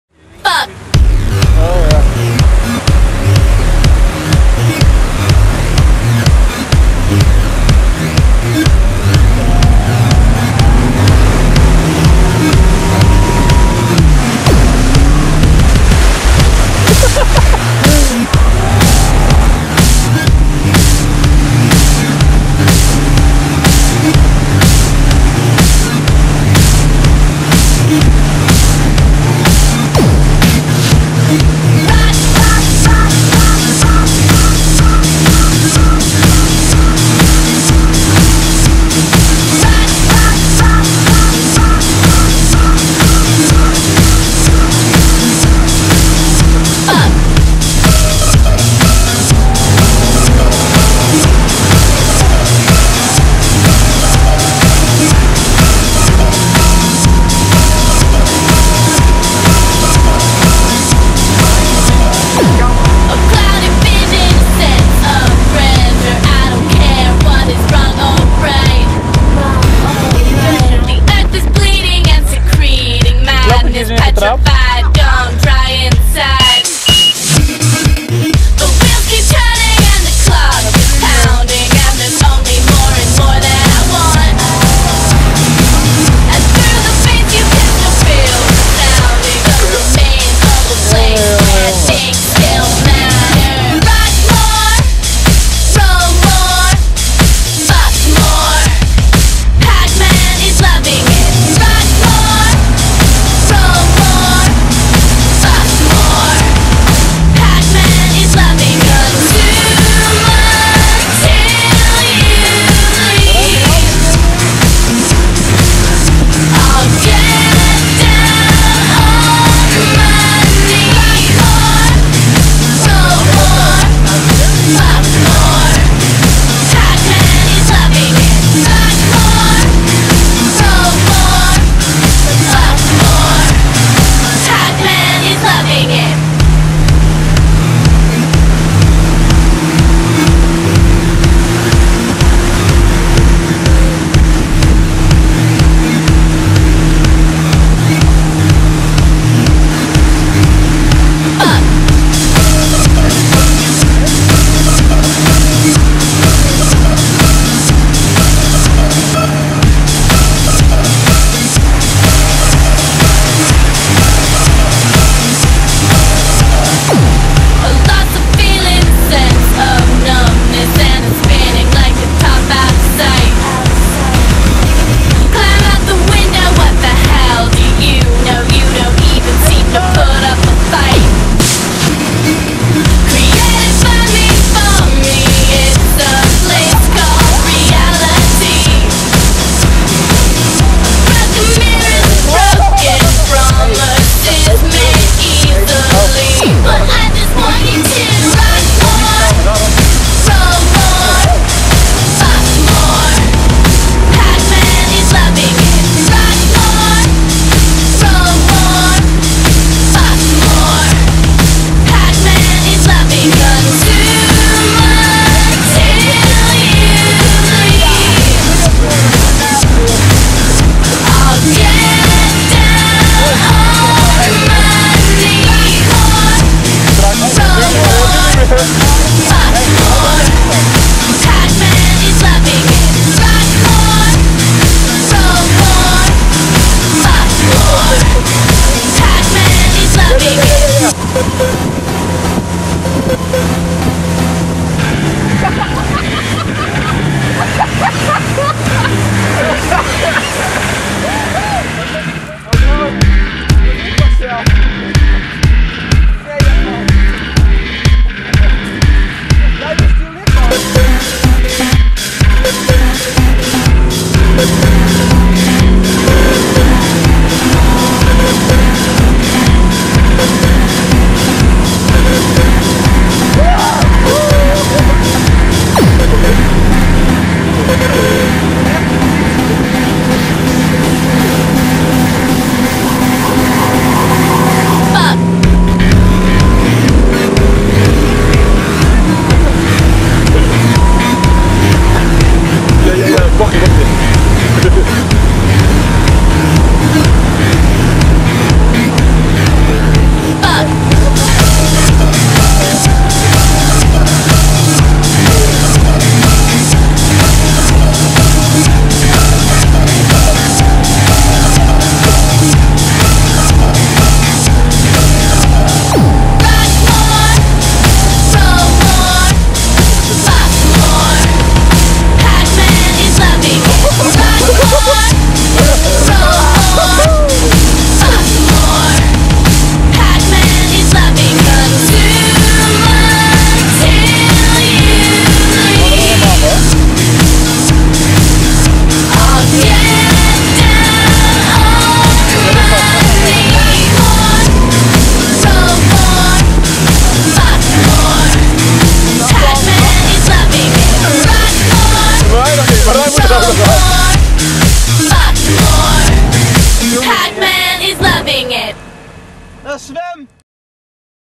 achter de boot
Op de banaan, kneeboard en band achter de boot